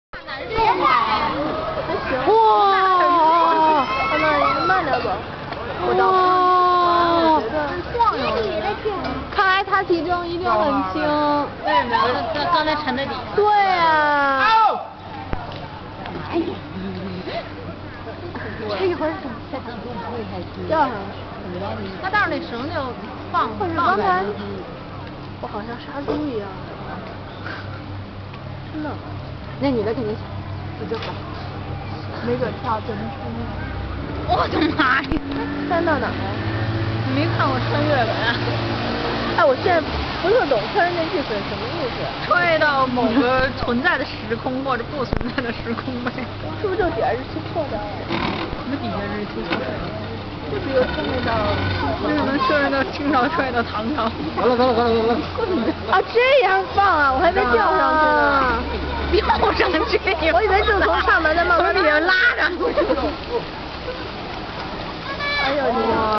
PHOEBE BUNGEE JUMPED!
MUAHAH my first bungee jumping ever. It was so much fun!